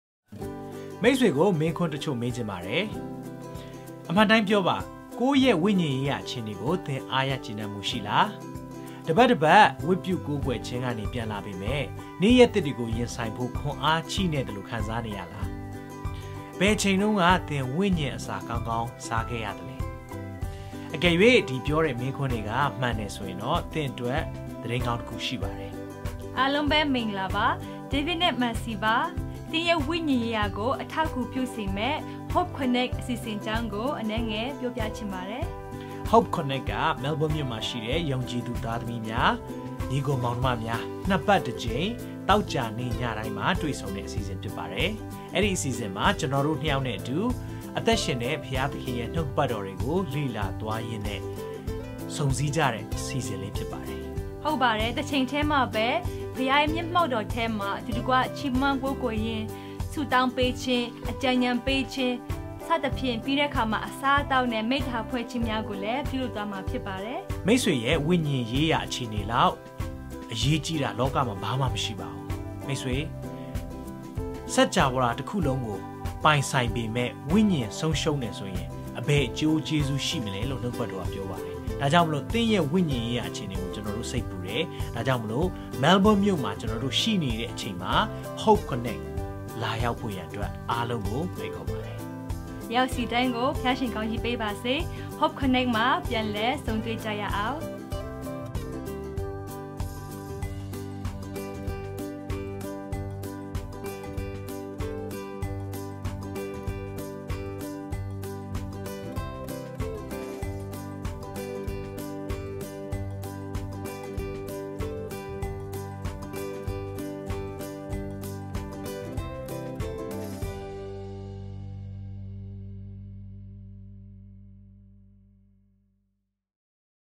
သင့်ဝိညာဉ်ရေးရာအတွက် | Hope Connect
မိတ်ဆွေ အမှန်အတိုင်းဖြေပါ

၁) သင့်ဝိညာဉ်ရေးရာ အခြေအနေကို အားရကျေနပ်မှုရှိလား?
၂) အပတ်စဉ် ဝတ်ပြုကိုးကွယ်ခြင်းအစီအစဉ်ကပြန်လာပေမယ့် နေ့ရက်သစ်တွေကို ရင်ဆိုင်ဖို့ ခွန်အား မရှိသလို ခံစားနေရလား?
၃) ဝိညာဉ်အစာကောင်းကောင်းကို ဘယ်တုန်းက နောက်ဆုံးစားခဲ့သလဲ?

သင့်ဝိညာဉ်ရေးရာလောက် လောကကြီးမှာ အရေးကြီးတာမရှိပါဘူး မိတ်ဆွေ။
ဒါကြောင့် သင့်ဝိညာဉ်ရေးရာအတွက် Hope Connect ကိုလာခဲ့ပါ။

Hope Connect ဆိုတာ Melbourne မှာ ရှိတဲ့ ယုံကြည်သူများ ၂ ပတ်တစ်ကြိမ် သောကြာနေ့တိုင်း တွေ့ဆုံတဲ့ နေရာလေးဖြစ်ပါတယ်။

Hope Connect မှာ ဆရာ David Kim နဲ့ ဆရာမ Mercy Kim တို့ထံမှ လတ်ဆတ်ကောင်းမွန်တဲ့ နုတ်ကပတ်တော်ကို ကြားရမှာ ဖြစ်ပါတယ်။

ဒါ့အပြင် ဘုရားရဲ့ မျက်မှောက်တော်ထဲကို ချီးမွမ်းကိုးကွယ်ခြင်းအားဖြင့် အတူအကွ တိုးဝင်မှာ ဖြစ်သလို ဘဝမှာ လိုအပ်တဲ့ အကြံဉာဏ်တွေလည်း ရရှိမှာ ဖြစ်ပါတယ်။

ဒါကြောင့် သင့်ဝိညာဉ်ရေးရာအတွက် Hope Connect ကို လာရောက်ဖို့ ဖိတ်ခေါ်ပါတယ်။

Next Meeting:
ဆရာ David Kim နေအိမ် ( လာရောက်လိုပါက လိပ်စာ ပို့ပေးမည်)
Friday 21 December 2018
6:30pm BBQ & Fellowship
7:30pm Worship